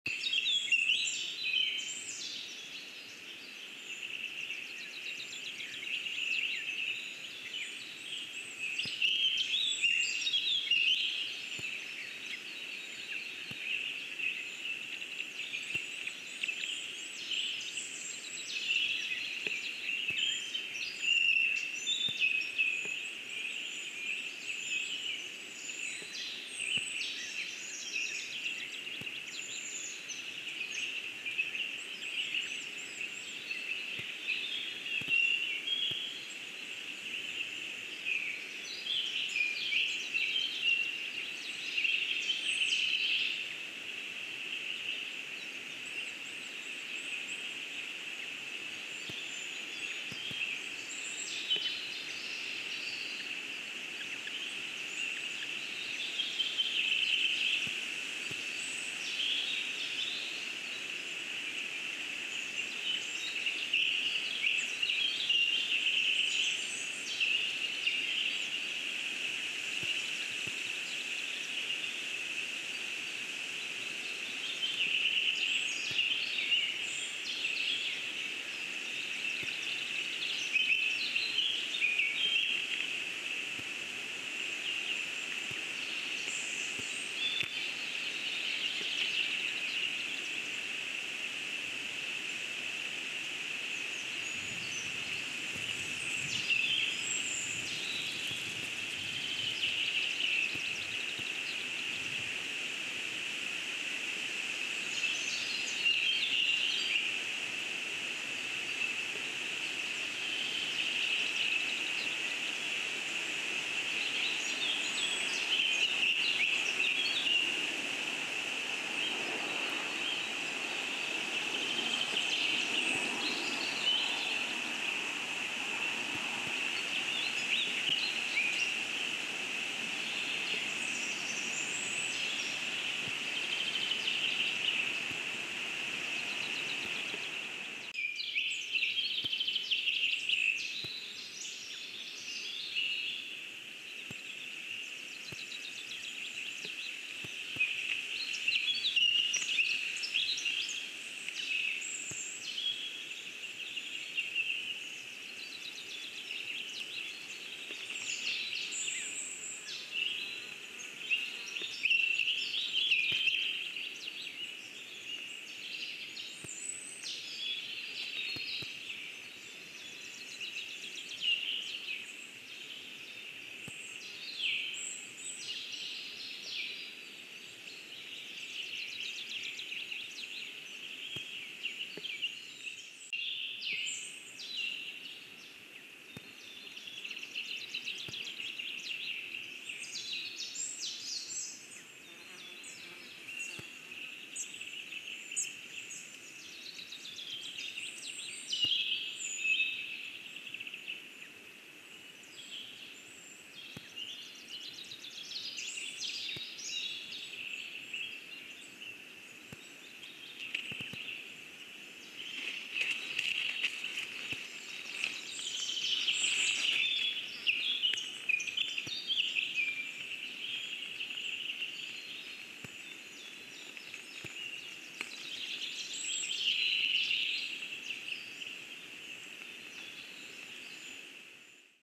LDF.Mamma sargā mazo no tveicīgās saules./Mom protects the little one from the hot sun.🥰11.06.23
LDF Mazais ērglis (Clanga pomarina), ligzda bērzā